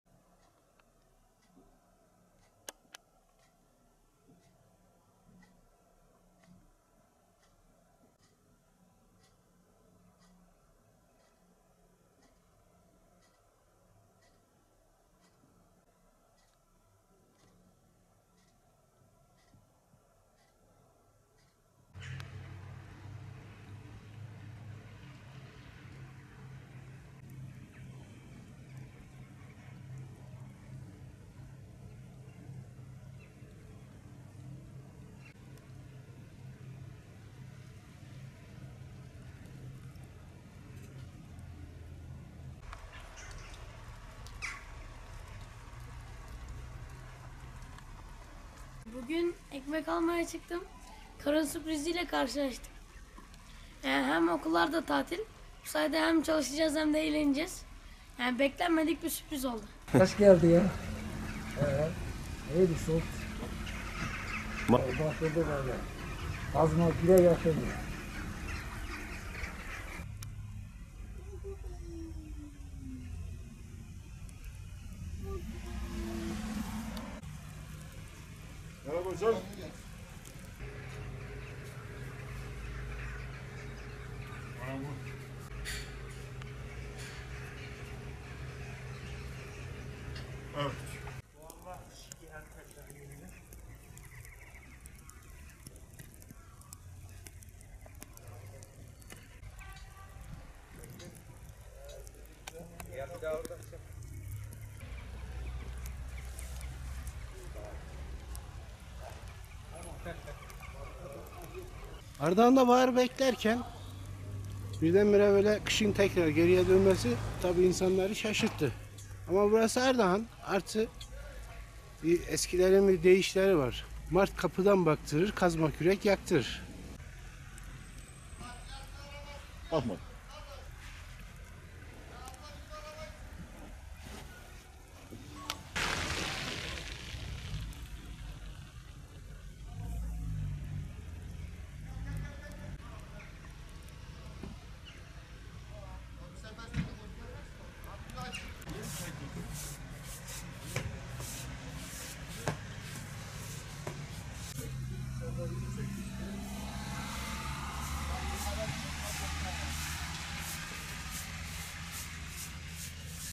Ardahan'da 'Mart Kapıdan Baktırır' Atasözü Gerçek Oldu, Lapa Lapa Kar Yağdı
Kar yağışının etkili olduğu Ardahan kent merkezinde ‘Mart kapıdan baktırır, kazma kürek yaktırır’ atasözü gerçek oldu.

Ardahan'da sabah saatlerinde başlayıp aralıklarla devam eden kar yağışı hayatı olumsuz etkiledi. Kent merkezinde gece başlayan kar yağışı şiddetini artırarak devam ediyor.
Geçtiğimiz günlerin güneşli olmasına rağmen güne karla uyandıklarını belirten Ardahanlılar bu duruma alışkın olduklarını söylediler.
Sabah kar sürpriziyle karşılaşan vatandaşlar ev ve iş yerlerinin önünde biriken karları ise küreklerle temizledi.